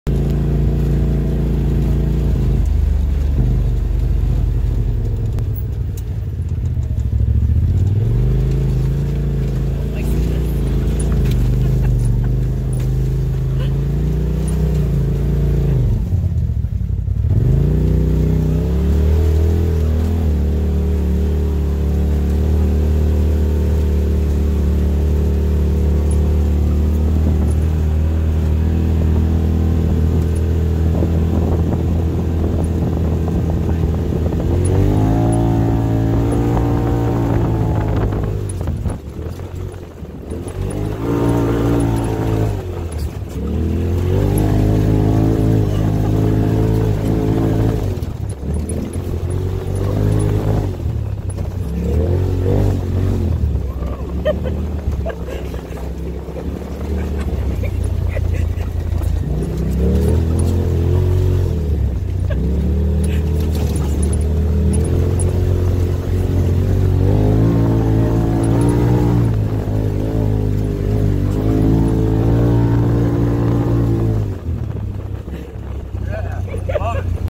Adrenaline Driven Adventures in Grand Junction
side by side Polaris RZR tour in Grand Junction, Colorado. A moment to take in the scenery. Ignore my nervous giggling.